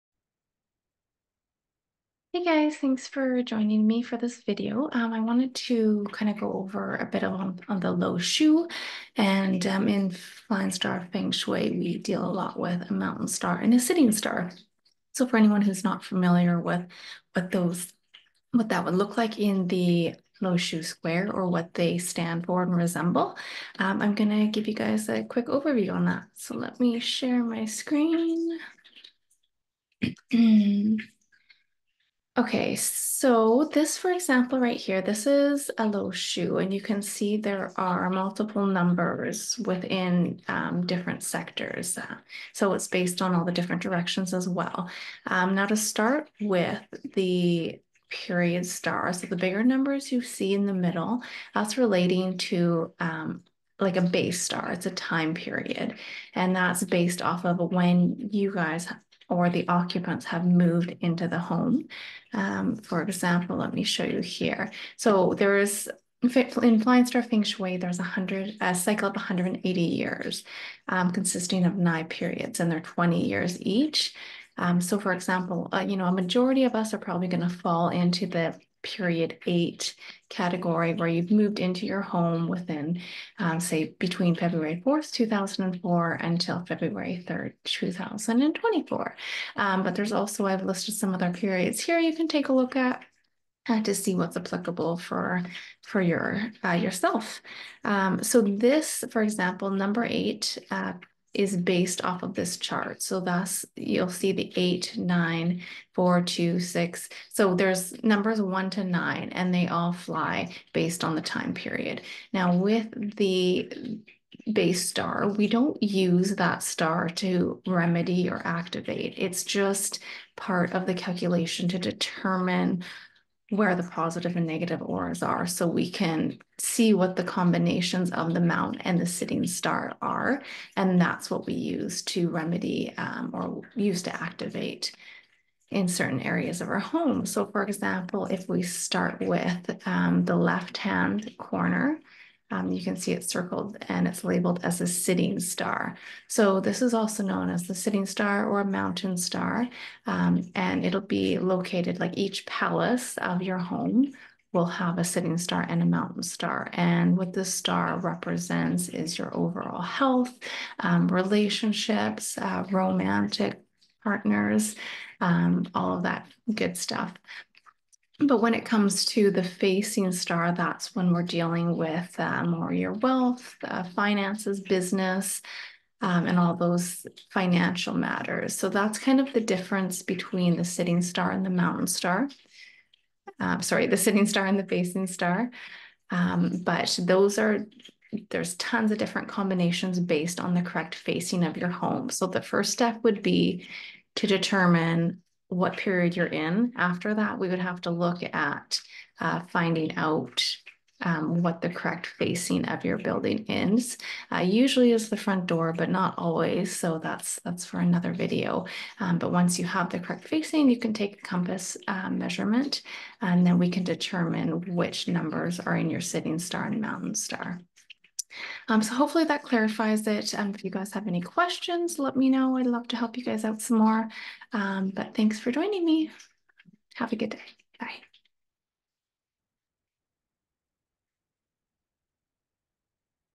Sitting Star and the Mountain Star - Classical Feng Shui
The Sitting star will be located in the top left box of the LuoShu and governs an occupants health, relationships with friends and family, and romances.
This star is activated with a YIN energy. it's a still and quiet, but yet big and stable energy For example a Mountain would activate these stars.

The Facing star is located on the top right hand corner of the LuoShu and governs an occupants wealth, finances and business matters.
This star is activated with a YANG energy.
(Its busy and constant movement will active these stars.) For example,  water features are a common and powerful way to active a facing star.  Also just occupying that space multiple hours a day, and having the TV or radio on for example will also activate this star.

MEMO: DO NOT PUT A WATER FEATURE IN YOUR HOME, UNLESS THE ENERGY SUPPORTS THIS!.  Water is an extremely powerful force to usher in certain energies, and that could bring in good or bad chi so Please seek guidance from a professional Feng Shui Consultant before installing one.